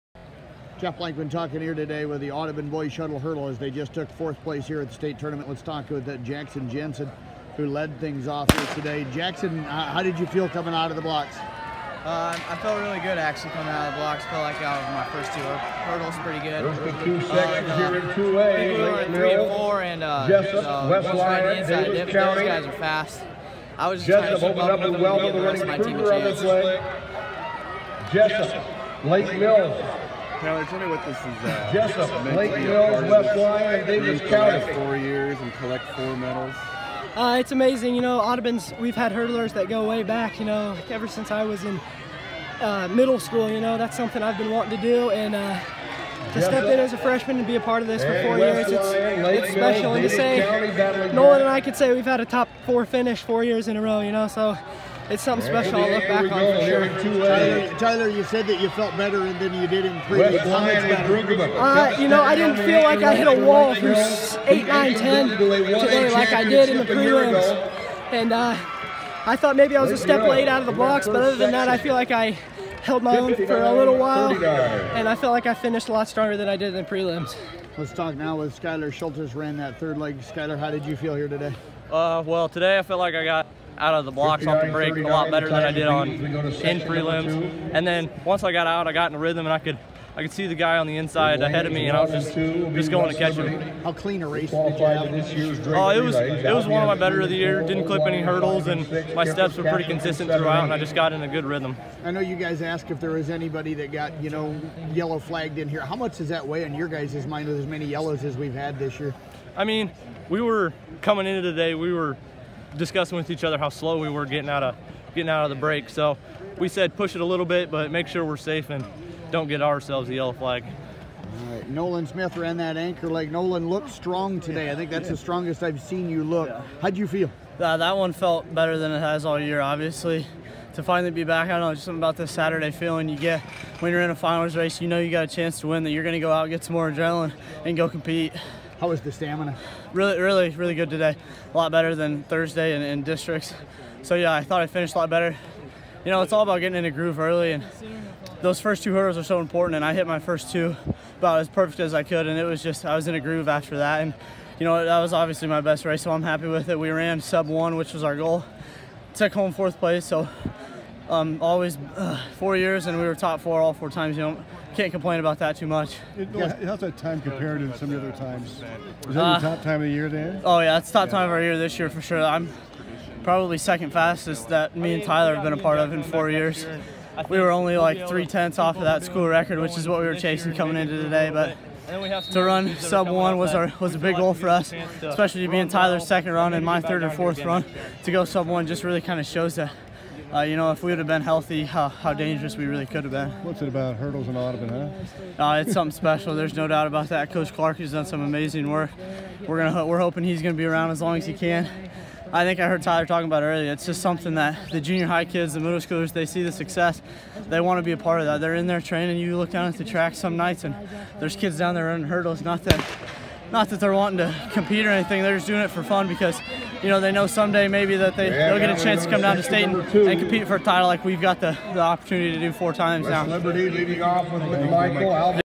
Jeff Blankman talks with Jackson Jensen, Tyler Riebhoff, Skyler Schultes and Nolan Smith
Audubon 4th place Shuttle Hurdle